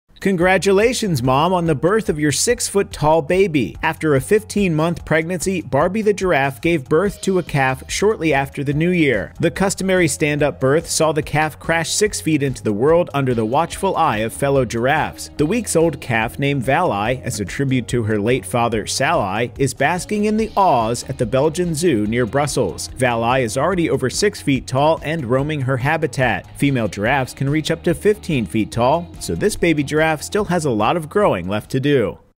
See the 6-Foot-Tall Baby! Don’t Worry, It’s a Cute Giraffe
After a 15-month pregnancy, Barbie the giraffe gave birth to a calf shortly after the New Year. Buzz60’s Justin Kircher has the story.